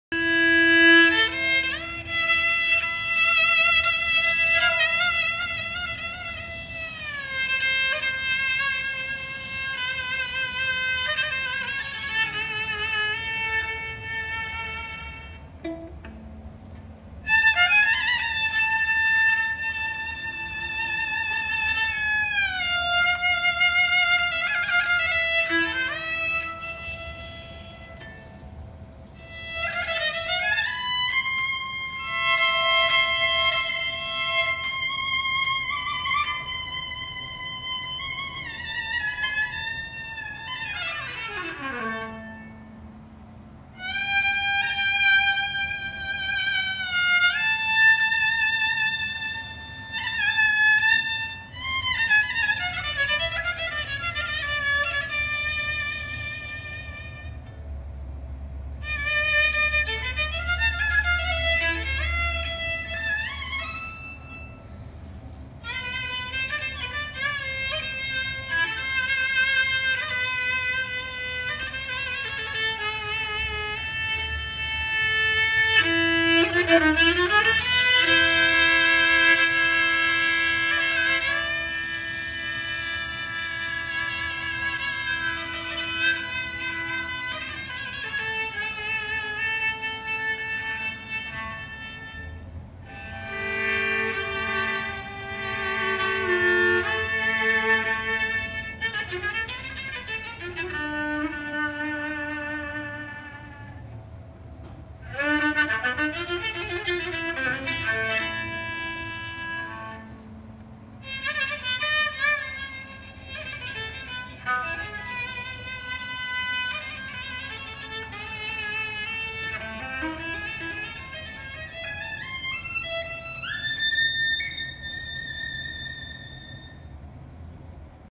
Maestro V Grigorian plays kamancha at Abril Bookstore
Sneak peak of Maestro V. Grigorian’s Kamancha concert at Abril Bookstore which will be on Saturday, October 27, 2018.